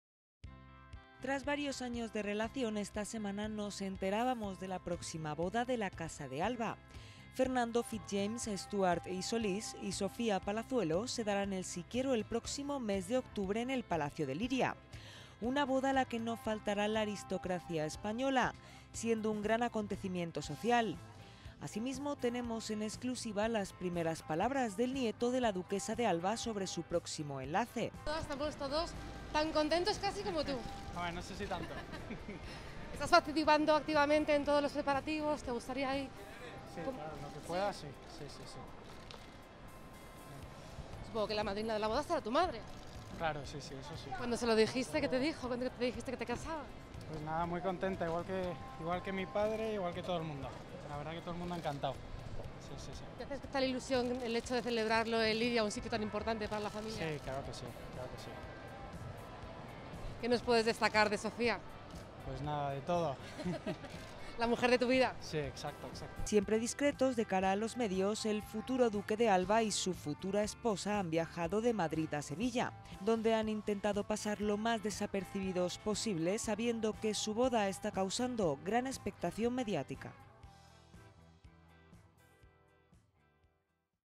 Primeras palabras de Fernando Fitz-James Stuart sobre su boda
Tras varios años de relación, esta semana nos enterábamos de la próxima boda de la Casa de Alba. Fernando Fitz-James Stuart y Solís y Sofía Palazuelo se darán el sí quiero el próximo mes de octubre en el Palacio de Liria. Una boda a la que no faltará la aristocracia española siendo un gran acontecimiento social. Asimismo tenemos en exclusiva las primeras palabras del nieto de la Duquesa de Alba sobre su próximo enlace.